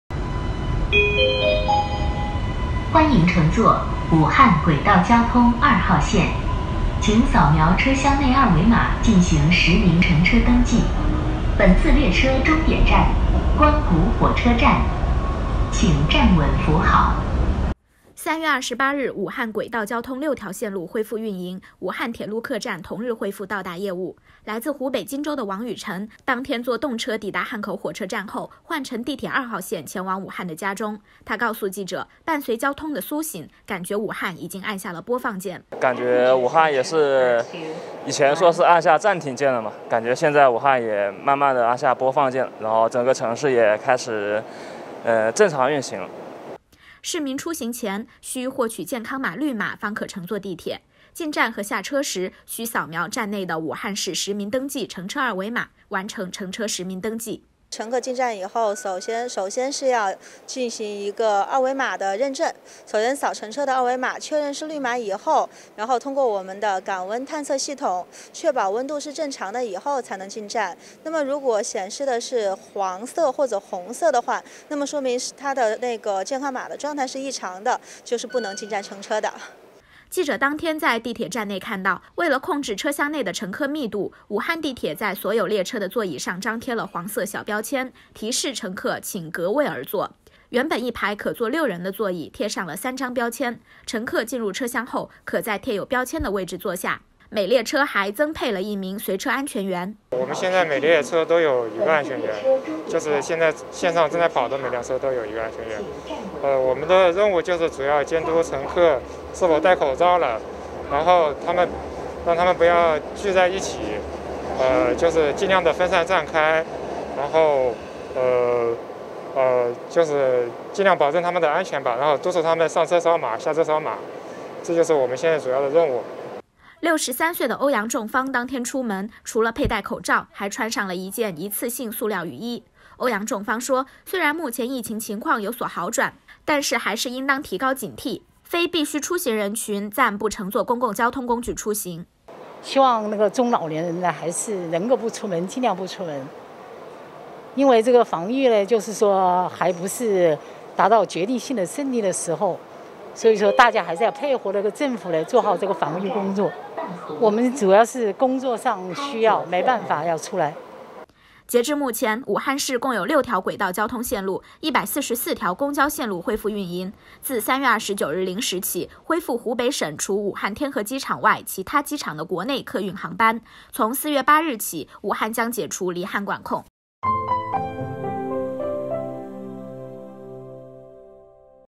武汉地铁恢复运营 市民：感觉城市按下了“播放键”
“欢迎乘坐武汉轨道交通2号线，请扫描车厢二维码… …”3月28日，武汉轨道交通6条线路恢复运营，武汉铁路客站同日恢复到达业务。来自湖北荆州的王雨尘，当天坐动车抵达汉口火车站后，换乘地铁2号线前往武汉的家中。他告诉记者，伴随交通的苏醒，感觉武汉已经按下了“播放键”。